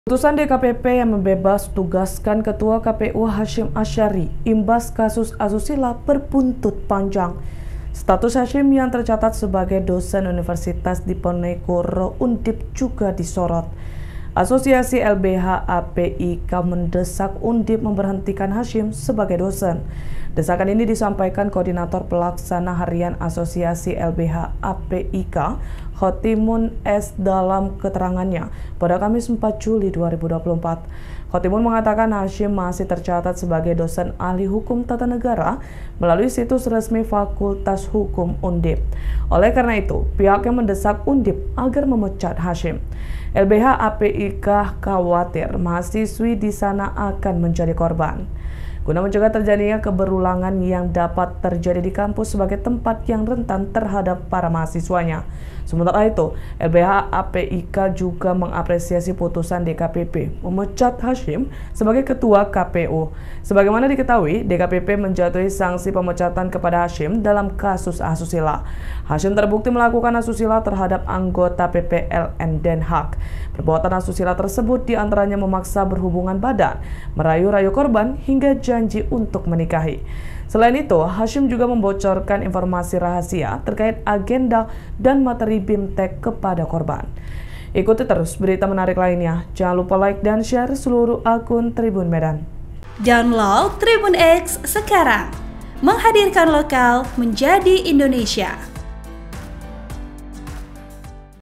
KARIR Hasyim Asy'ari Runtuh, Bakal Dipecat Undip dari Dosen, LBH APIK Khawatir Mahasiswa Jadi Korban
TRIBUN-MEDAN.COM - Putusan DKPP yang membebastugaskan Ketua KPU Hasyim Asy'ari imbas kasus asusila berbuntut panjang.

Status Hasyim yang tercatat sebagai dosen Universitas Diponegoro (Undip) juga disorot.

Asosiasi LBH APIK mendesak Undip memberhentikan Hasyim sebagai dosen.

Desakan ini disampaikan Koordinator Pelaksana Harian Asosiasi LBH APIK, Khotimun S dalam keterangannya Kamis (4/7).

Khotimun mengatakan Hasyim masih tercatat sebagai Dosen Ahli Hukum Tata Negara melalui situs resmi Fakultas Hukum Undip.

Oleh karena itu pihaknya mendesak Undip agar memcat Hasyim.

LBH APIK khawatir mahasiswi di sana akan menjadi korban.

“Guna mencegah terjadinya keberulangan yang dapat terjadi di kampus sebagai tempat yang rentan terhadap para mahasiswinya,” ujarnya.

Sementara itu, LBH APIK juga mengapresiasi putusan DKPP memecat Hasyim sebagai Ketua KPU.

Sebagaimana diketahui, DKPP menjatuhi sanksi pemecatan kepada Hasyim dalam kasus asusila.

Hasyim terbukti melakukan asusila terhadap anggota PPLN Den Haag.

Perbuatan asusila tersebut di antaranya memaksa berhubungan badan, merayu-rayu korban, hingga janji untuk menikahi.

Selain itu, Hasyim juga membocorkan informasi rahasia terkait agenda dan materi bimtek kepada korban

Host : Mollyzatul
Editor Video : Jihan Fikriyah